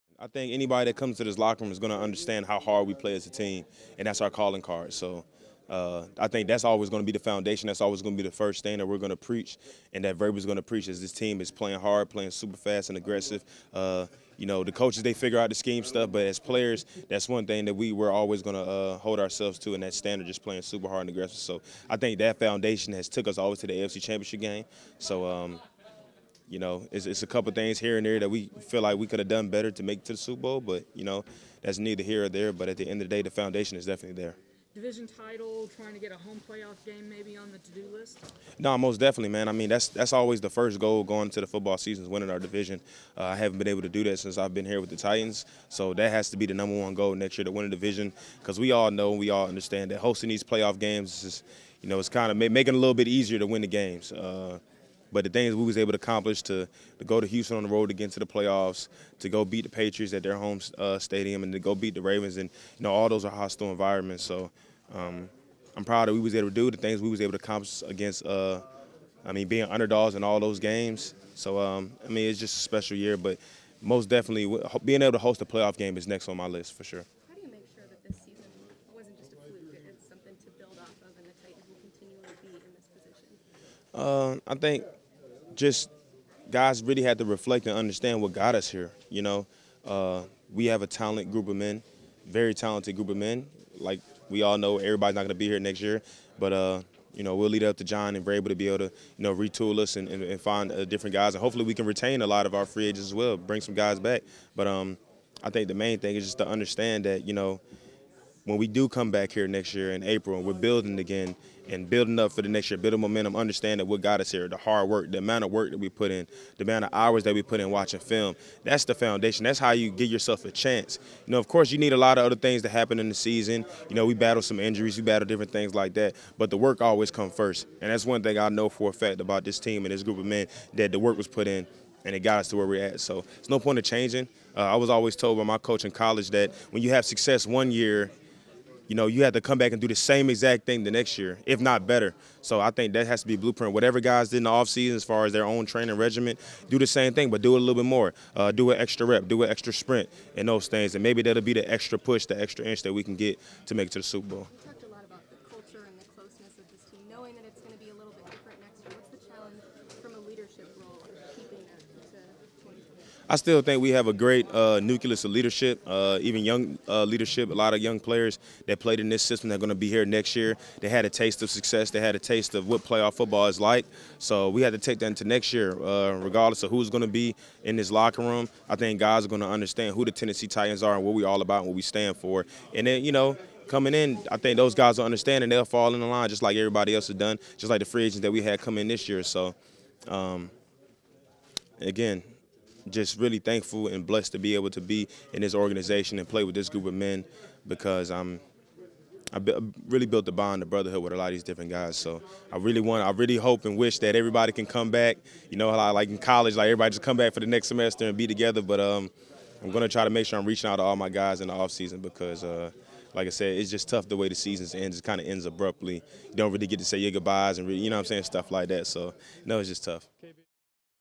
Kevin Byard: The Foundation is Definitely There
Titans safety Kevin Byard speaks with reporters Monday at Saint Thomas Sports Park.